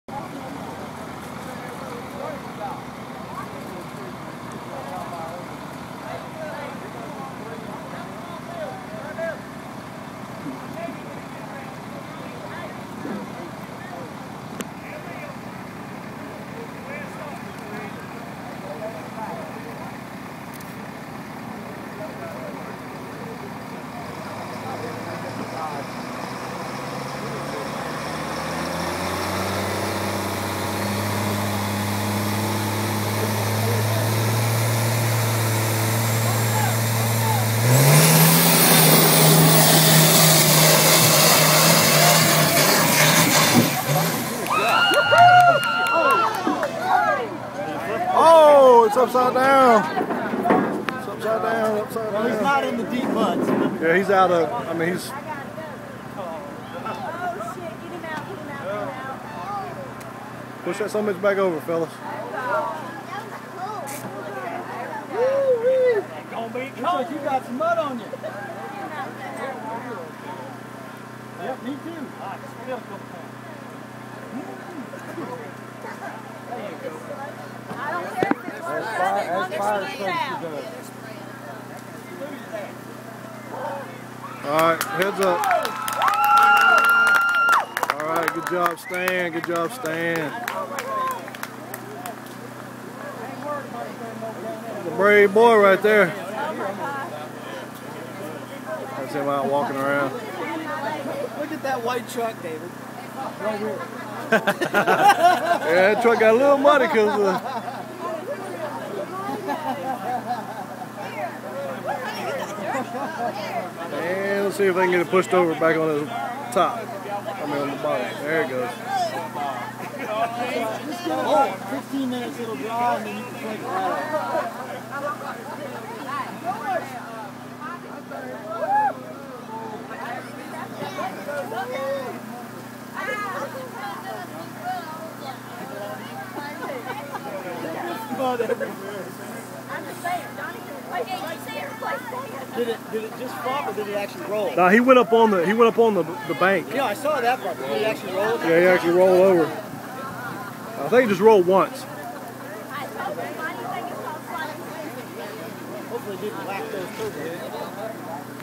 Slighly Stupid Mud Cricket Rolled
Stan in the Mud Cricket going for a wild ride!  Hlad you're okay Buddy!